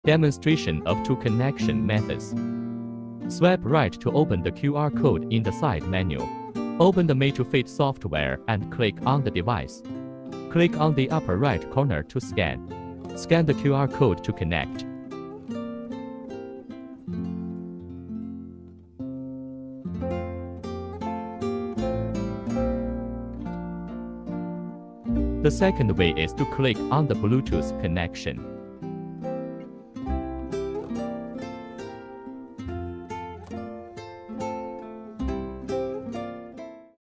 ⌚MT78 ULTRA Music Connection Video
Upgrade highlights😍:
1. 2.2-inch large screen
2. Resolution 485*520 display more clearly
3. Added siren to make you safer outdoors
4. Custom music background, custom dial, DIY LOGO more personalized
5. The custom operation of the third button is more convenient
6. The newly revised UI gives you a smoother experience
7.Play local music, leave your phone, listen to it whenever you want